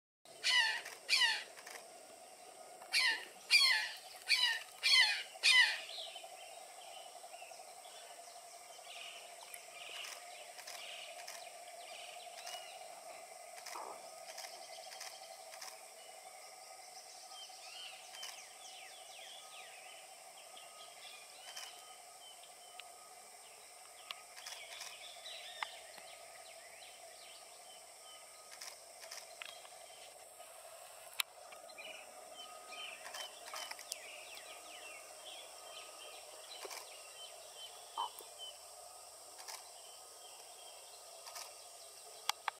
Brown Jay in South Texas
Exciting birds discovered at Santa Margarita Ranch near Salineno, Texas!

This is a private ranch, but if you want to see the jays for yourself check out the info